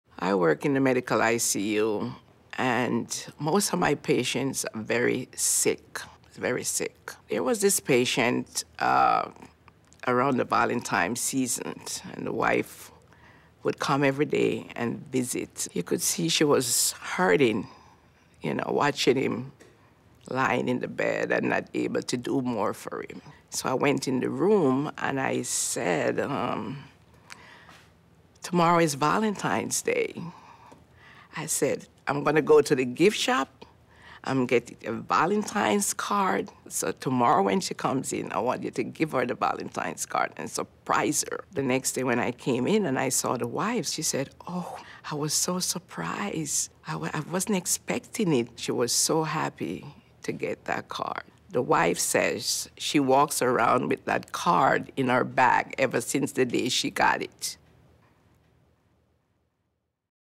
Jean Miller's Story - NewYork-Presbyterian
Long hospital stays are tough not only on patients but on their families, too. Schedules get disrupted, holidays go uncelebrated. Jean Miller, a unit clerk at NewYork-Presbyterian, decided to do something about it by buying a Valentine's Day card for a very sick patient to give to his wife.

Jean wasn't sure she wanted her story turned into a TV commercial. The way she sees it, thinking about what patients and their families need is part of her job description and what she did for this patient "was no big thing." Her patients and co-workers disagree.